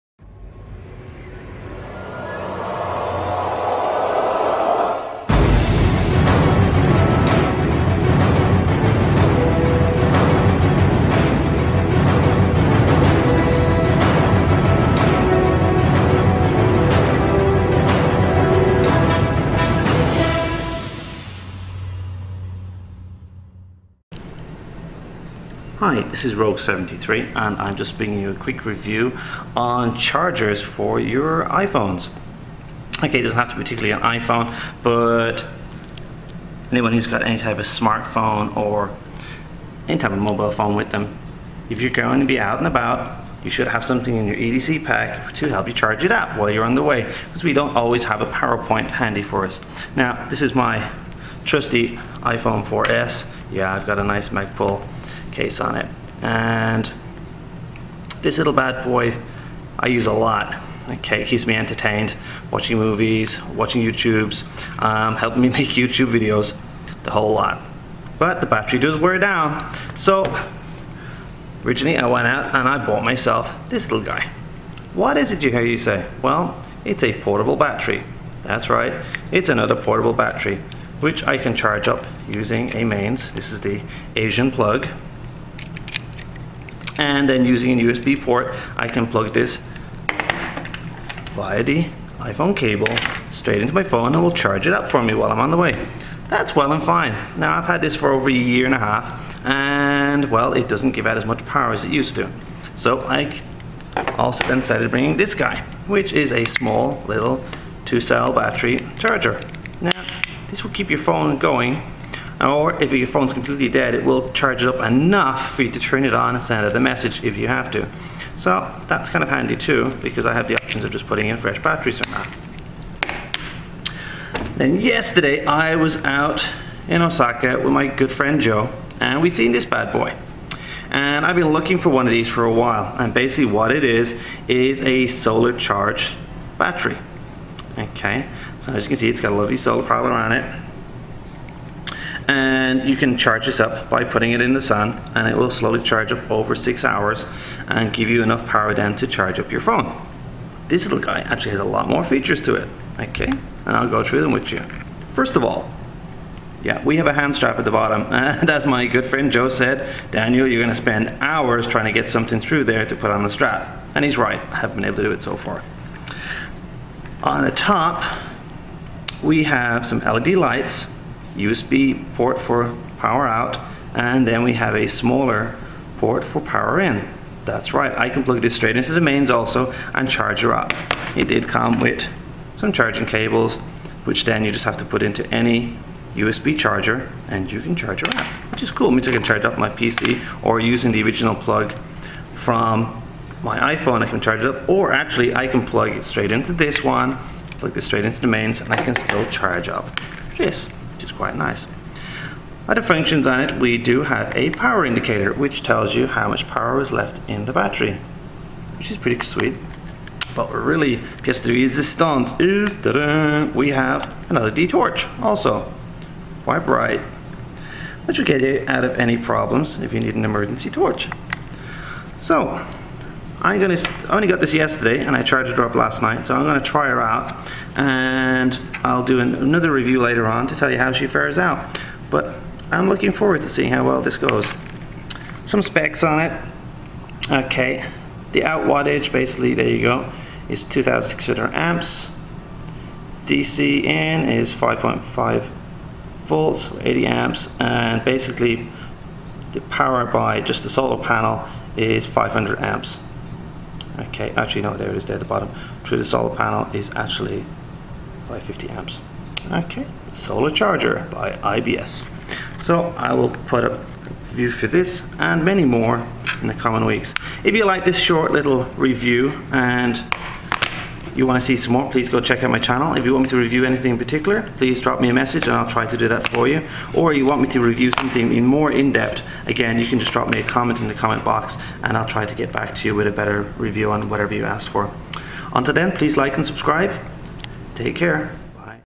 Solar Charger
Here is another quick review on my new
2600mAh Solar Charger.
This has a built in LED light and battery meter. Can be charged up by mains ,USB mini or by the sun.